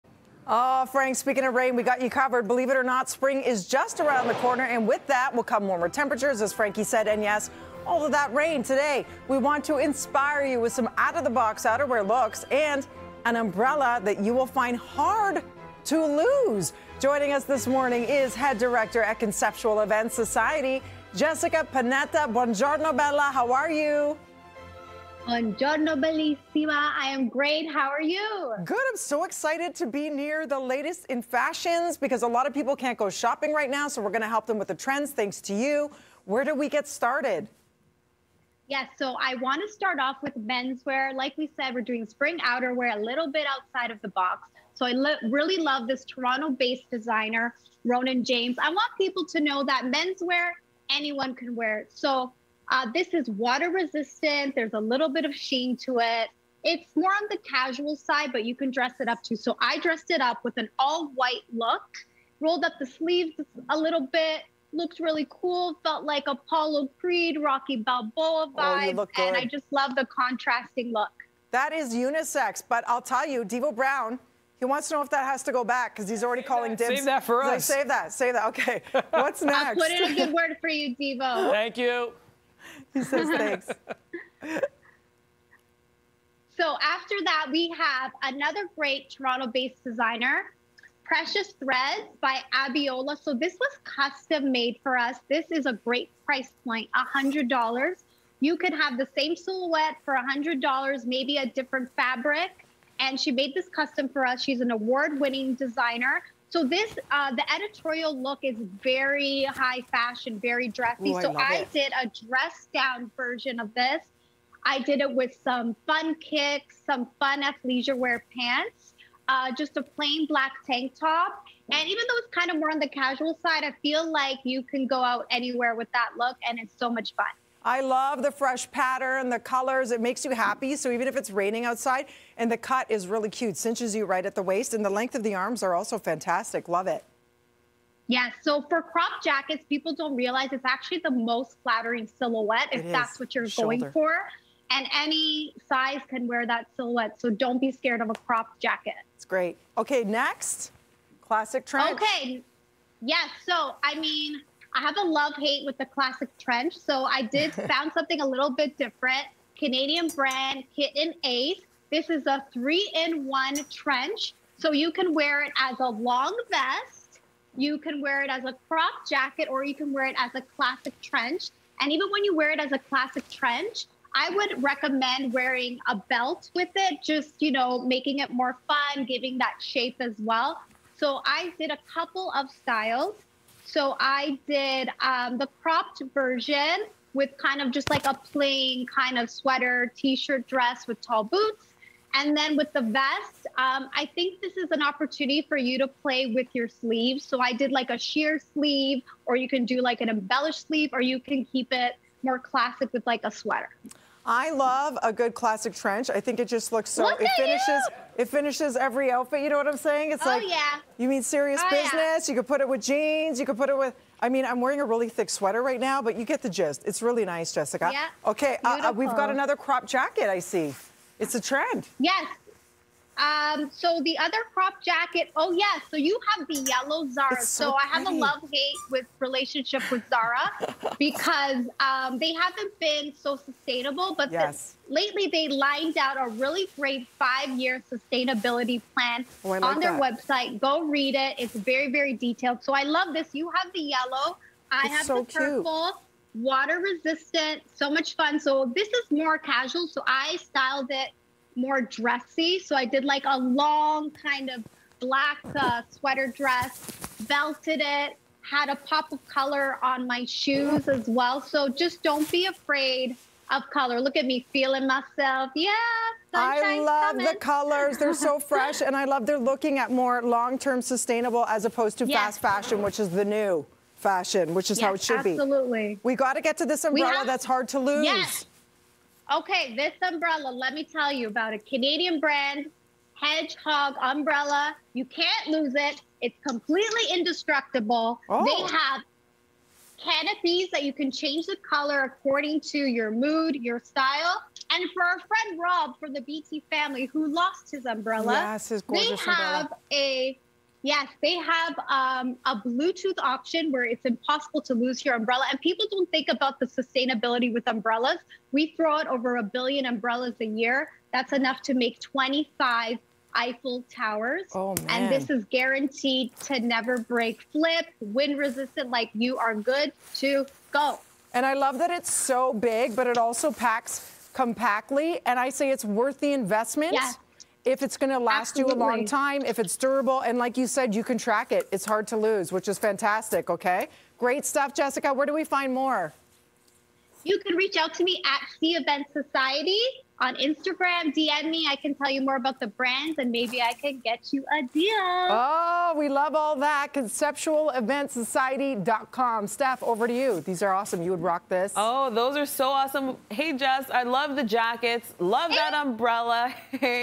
Check Out These Out of the Box Outerwear Looks for the Spring
Dina chats with Head Director at Conceptual Events Society Jessica Panetta about some out of the box outerwear looks and an umbrella that you will find HARD to lose!